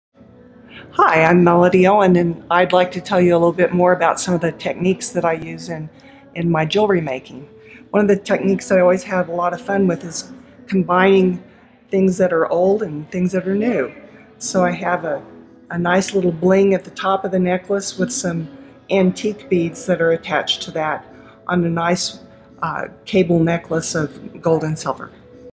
Melodie Owen Mixing Old with the New
Melodie Owens is dedicated to synthesizing techniques and materials in new ways. Being open to new ideas and techniques is what keeps her designs and process stimulating her work continually evolves.

She works with many metals and adds color using gems, crystals, fused glass,  fibers, and found objects. Her work is handcrafted personally in her High Desert Albuquerque studio.

Melodie Owen Designs can be found:
Gee Loretta!

Mon- Sat 10-6